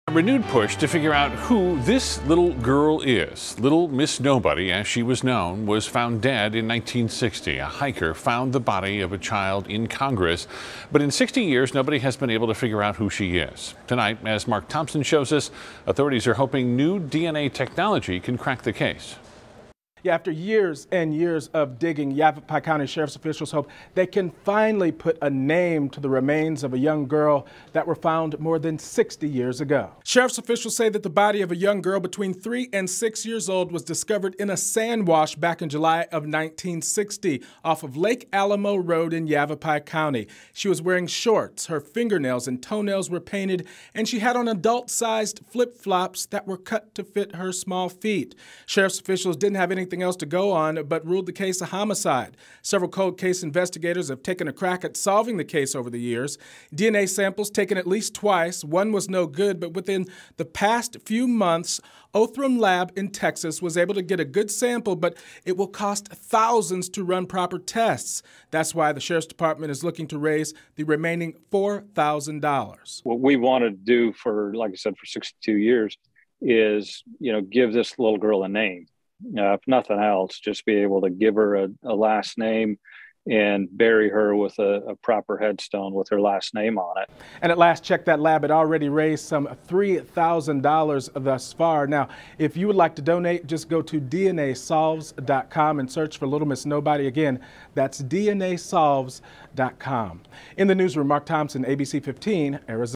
Fundraiser hopes to help ID 'Little Miss Nobody' found dead in Arizona in 1960
The remains of “Little Miss Nobody” were discovered in the desert in 1960, and today officials are hoping to raise money to help identify the little girl.